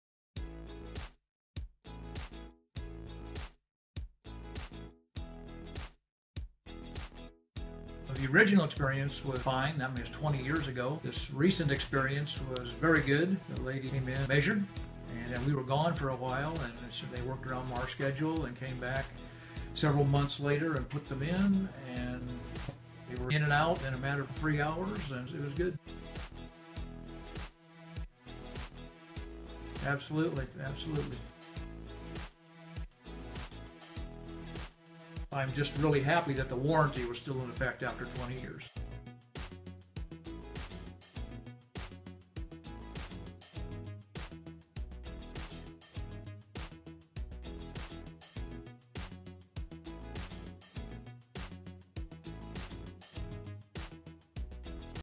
Bee Window - Schemehorn Family Testimony (Bow Window Installation)
What a cool guy who invited us into his neat home to talk about his personal experience with Bee!!